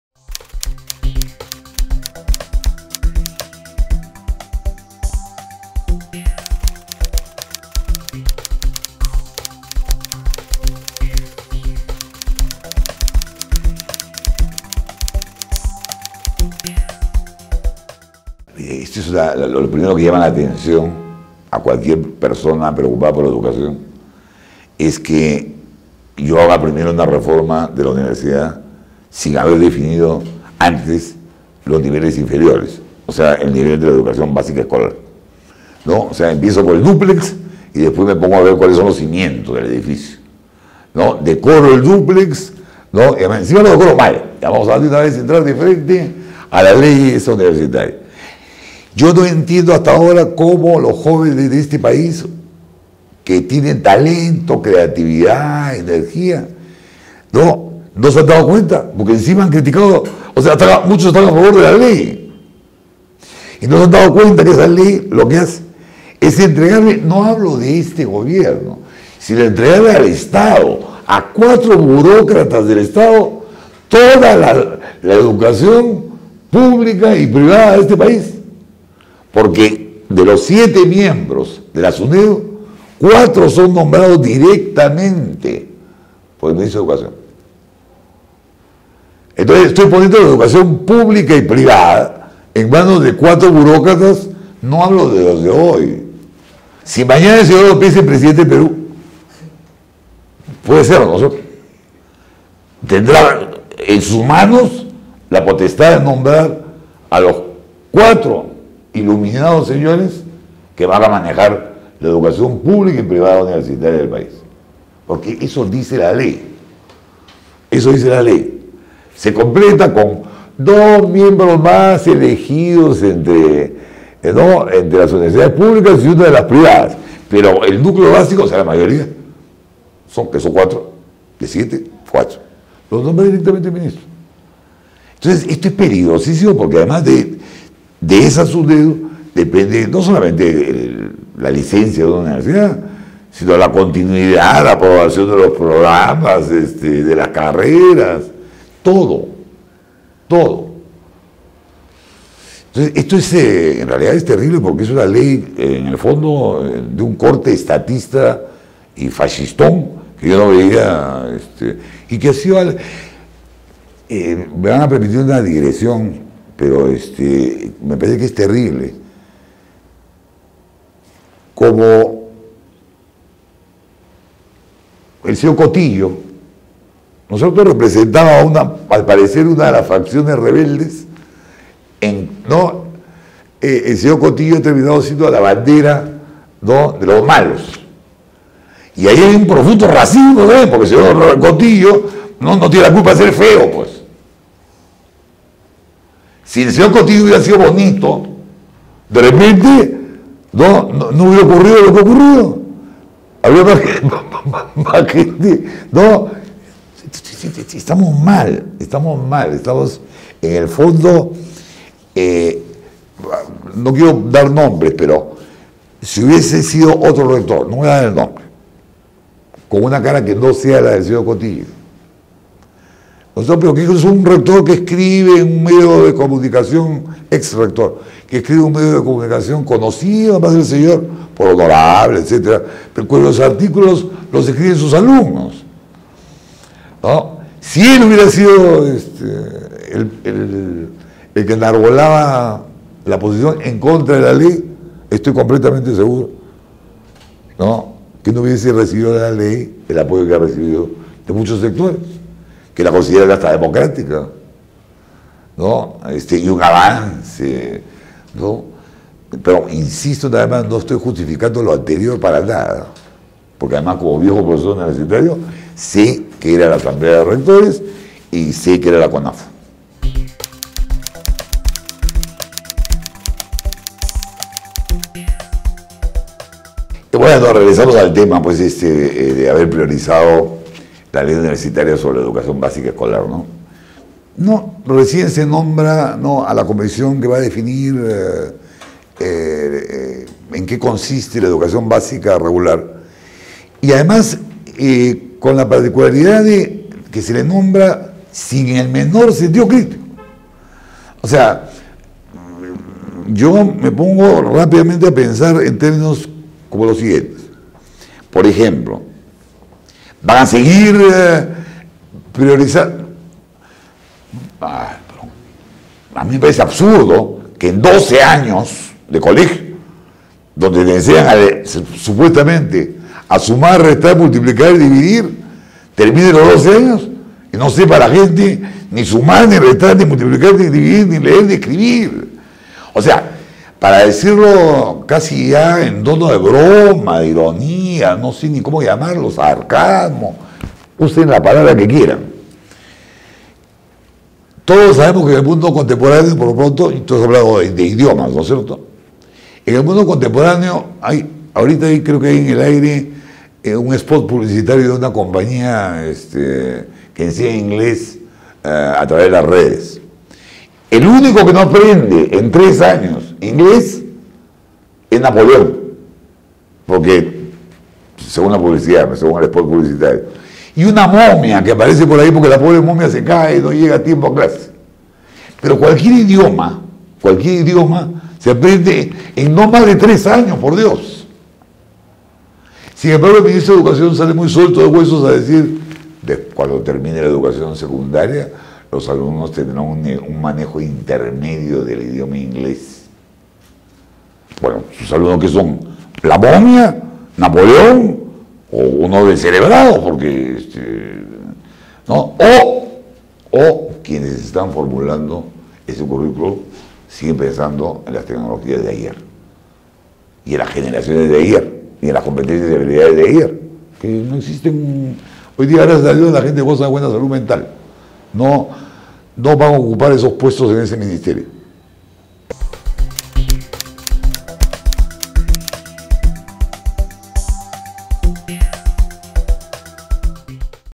Ley universitaria institucionaliza mediocridad
El lingüista y semiólogo Eduardo Zapata reflexiona sobre la ley universitaria, los problemas en la educación básica regular  y las debilidades para lograr una educación superior.

Lee, observa, escucha y difunde.